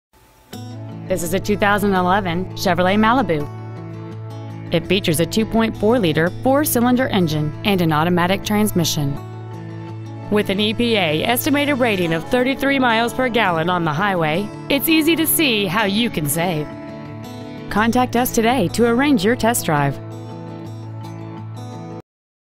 2011 Chevrolet Malibu Chicago IL
Year : 2011
 Make : Chevrolet
 Model : Malibu
 Engine : Ecotec 2.4L DOHC 16-Valve 4-Cyl VVT MFI Engine
 Trans . : Automatic
 Exterior : Unspecified
 Miles : 29,925
 Interior : Unspecified

 2011 Chevrolet Malibu Hodgkins IL